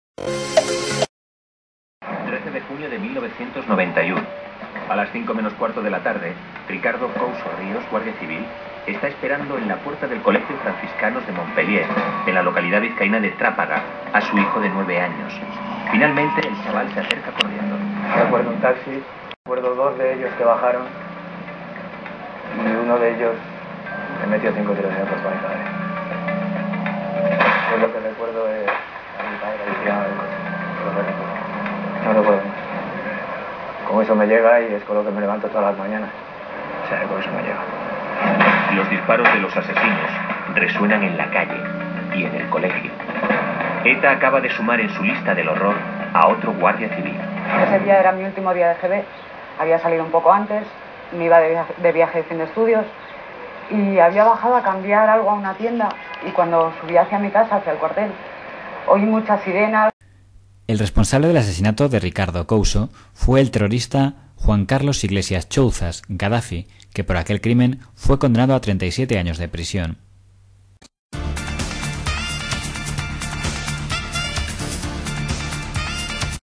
1991- Asesinato de Ricardo Cousso
Víctima: Ricardo Cousso
Asesinos: Juan Carlos Iglesias Chouzas 'Gadafi', Jesús María Mendinueta y Juan María Ormazabal 'El Turco'.